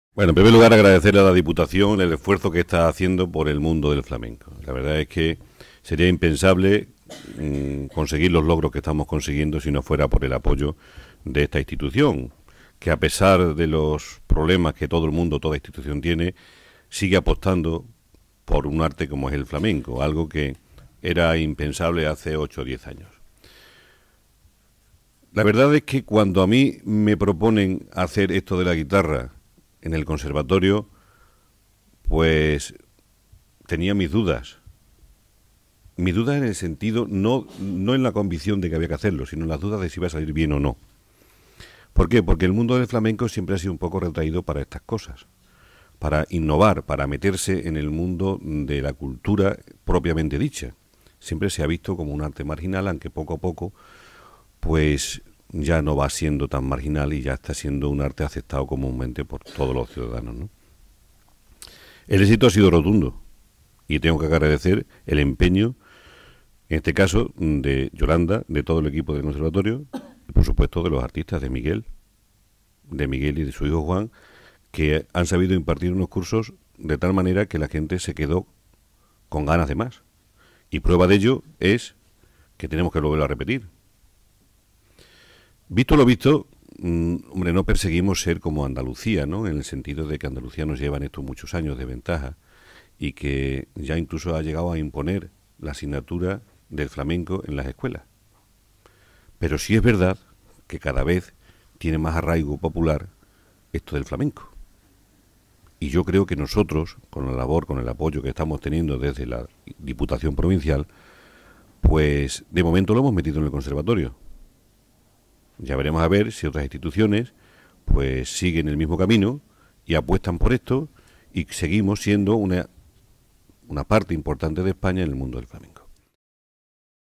Cursos de guitarra flamenca en el Conservatorio Juan Vázquez / Celestino Vegas
Después del éxito cosechado por el curso anterior, celebrado durante el pasado trimestre en el Conservatorio Profesional de Música "Juan Vázquez", se ha organizado una segunda edición  sobre la guitarra flamenca que se va a desarrollar a partir del próximo 15 de febrero y hasta finales de mayo en las mismas instalaciones del centro de formación musical.

     El diputado-delegado del Área de Cultura, Miguel Ruíz, ha presentado el nuevo curso recordando que "los grandes intérpretes flamencos, especialmente en el apartado de la guitarra, han ido ocupando espacios reservados desde siempre para lo clásico. De esta manera han contribuído a prestigiar los distintos aspectos de un arte que ha sido declarado Patrimonio Inmaterial de la Humanidad y que en Extremadura ha contado y cuenta con grandes artistas".

     Además de cantaores, la región posee excelentes "tocaores", guitarristas como Manolo de Badajoz y sus hermanos que han sido objeto de estudio en el libro "La barbería de las sonantas" de Manuel Iglesias y que editó la Diputación pacense.

    "Otra saga importante -añade Miguel Ruíz- es la conocida familia Vargas. Miguel, el padre, es un excelente guitarrista que domina todos los palos del flamenco, incluídos los tangos y jaleos que ha innovado mezclando el flamenco y el fado, el denomiando flamenfado que ha tocado junto a la Orquesta de Extremadura. Además,  Miguel Vargas ha acompañado a grandes artistas como Camarón de la Isla".

   Por tanto, nadie mejor que Miguel Vargas y su hijo Juan para repetir como profesor en esta segunda edición del curso de guitarra flamenca. Así lo corrobora el diputado provincial, explicando que después del primero, que contó con 20 alumnos más 10 oyentes,  este nuevo curso se impartirá todos los viernes desde el 15 de febrero y hasta finales del mes de mayo en horario de 18 a 20 horas".

     La directora del Conservatorio Profesional, Yolanda Sánchez, reconoce que ahora habla desde la tranquilidad tras el éxito alcanzado en aquella primera experiencia, pionera en Extremadura, tanto por el interés del alumnado como por la calidad del curso.